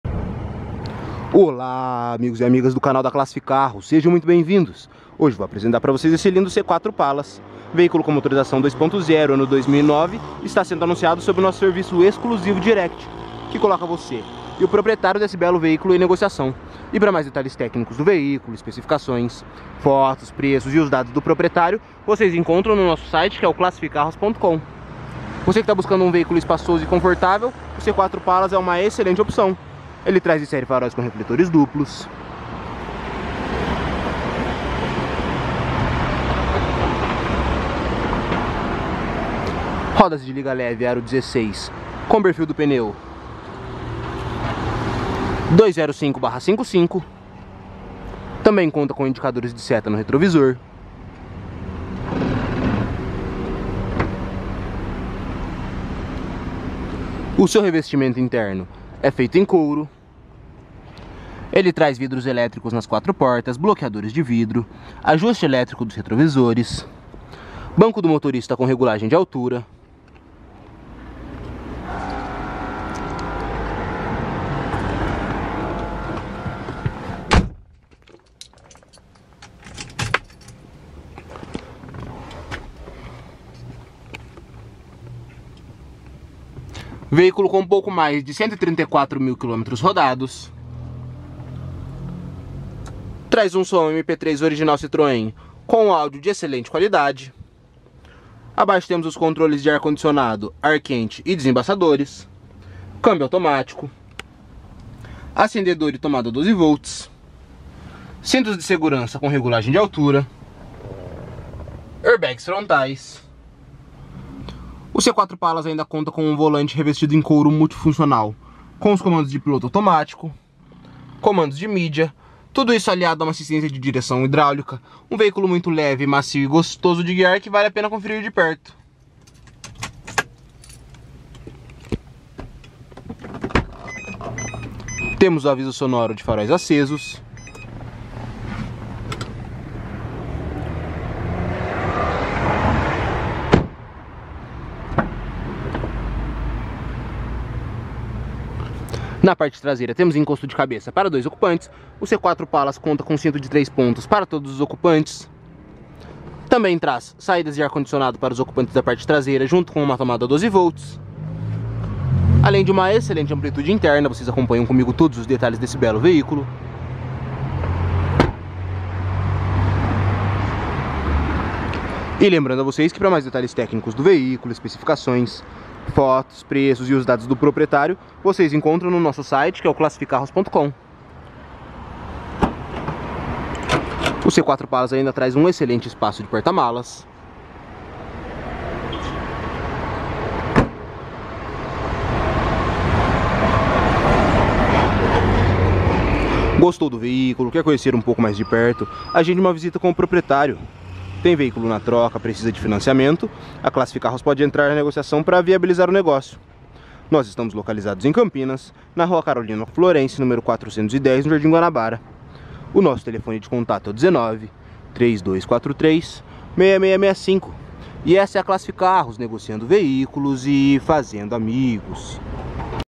Citroen C4 Pallas GLX 2.0 Automático 2009
▬ Confira a disponibilidade, preço e condições de negociação em nosso

NOVO. O C4 PALLAS É MUITO ELOGIADO PELO SEU ÓTIMO ESPAÇO INTERNO E OPCIONAIS DE ÓTIMA QUALIDADE. VERSÃO GLX CONTA C/ MOTOR 2.0 (151 cv), AIRBAG, FREIOS ABS, RODAS DE LIGA LEVE, AR CONDICIONADO, SOM MP3, COMPUTADOR DE BORDO E PT MALAS DE 580 L.